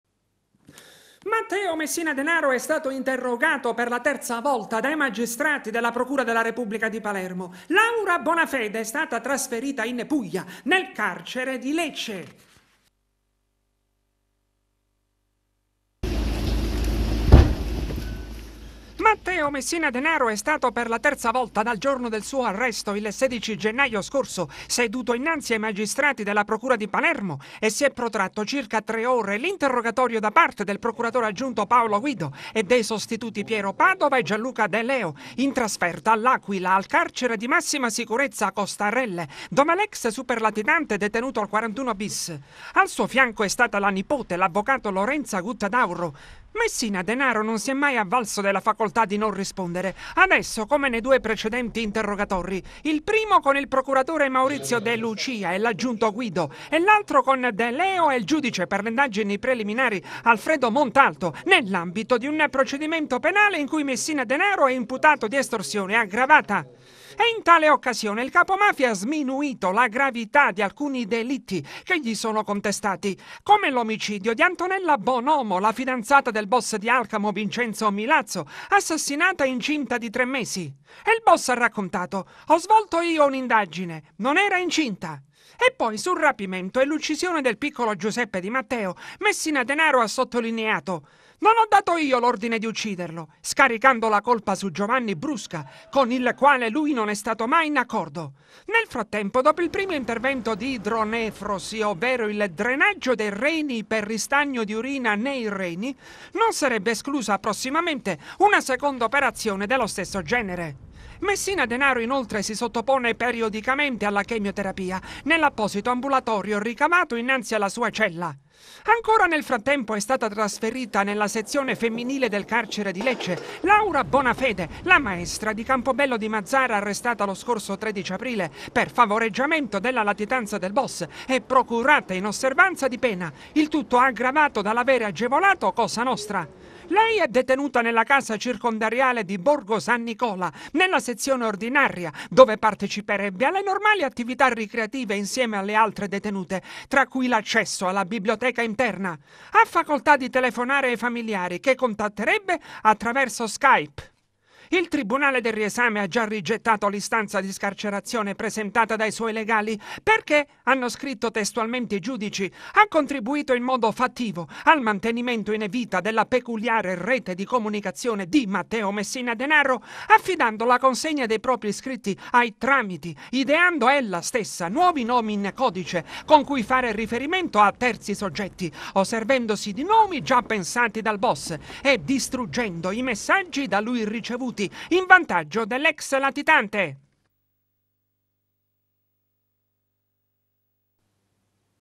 Ruoppolo Teleacras - Matteo Messina Denaro interrogato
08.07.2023  Matteo Messina Denaro è stato interrogato per la terza volta dai magistrati della Procura di Palermo. Laura Bonafede è stata trasferita in Puglia, nel carcere di Lecce. Il servizio di Angelo Ruoppolo.

Matteo Messina Denaro è stato per la terza volta dal giorno del suo arresto, il 16 gennaio scorso, seduto innanzi ai magistrati della Procura di Palermo. E si è protratto circa tre ore l’interrogatorio da parte del procuratore aggiunto, Paolo Guido, e dei sostituti Piero Padova e Gianluca De Leo, in trasferta a L’Aquila, nel carcere di massima sicurezza “Costarelle”, dove l’ex superlatitante è detenuto al 41 bis. Al suo fianco è stata la nipote, l’avvocato Lorenza Guttadauro. Messina Denaro non si è mai avvalso della facoltà di non rispondere, adesso come nei due precedenti interrogatori: il primo con il procuratore Maurizio De Lucia e l’aggiunto Guido, e l’altro con De Leo e il giudice per le indagini preliminari, Alfredo Montalto, nell’ambito di un procedimento penale in cui Messina Denaro è imputato di estorsione aggravata.  E in tale occasione il capomafia ha sminuito la gravità di alcuni delitti che gli sono contestati, come l’omicidio di Antonella Bonomo, la fidanzata del boss di Alcamo, Vincenzo Milazzo, assassinata incinta di tre mesi. E il boss ha raccontato: “Ho svolto io un’indagine, non era incinta”. E poi, sul rapimento e l’uccisione del piccolo Giuseppe Di Matteo, Messina Denaro ha sottolineato: “Non ho dato io l’ordine di ucciderlo”, scaricando la colpa su Giovanni Brusca, con il quale lui non è stato mai in accordo. Nel frattempo, dopo il primo intervento di idronefrosi, ovvero il drenaggio dei reni per ristagno di urina nei reni, non sarebbe esclusa prossimamente una seconda operazione dello stesso genere. Messina Denaro, inoltre, si sottopone periodicamente alla chemioterapia, nell’apposito ambulatorio ricavato innanzi alla sua cella. Ancora nel frattempo è stata trasferita nella sezione femminile del carcere di Lecce Laura Bonafede, la maestra di Campobello di Mazara arrestata lo scorso 13 aprile per favoreggiamento della latitanza del boss e procurata inosservanza di pena, il tutto aggravato dall’avere agevolato Cosa Nostra. Lei è detenuta nella casa circondariale di Borgo San Nicola, nella sezione ordinaria, dove parteciperebbe alle normali attività ricreative insieme alle altre detenute, tra cui l’accesso alla biblioteca interna. Ha facoltà di telefonare ai familiari, che contatterebbe attraverso Skype. Il Tribunale del Riesame ha già rigettato l’istanza di scarcerazione presentata dai suoi legali, “perché – hanno scritto testualmente i giudici –  ha contribuito in modo fattivo al mantenimento in vita della peculiare rete di comunicazione di Matteo Messina Denaro, affidando la consegna dei propri scritti ai tramiti, ideando ella stessa nuovi nomi in codice con cui fare riferimento a terzi soggetti o servendosi di nomi già pensati dal boss, e distruggendo i messaggi da lui ricevuti in vantaggio dell’ex latitante”.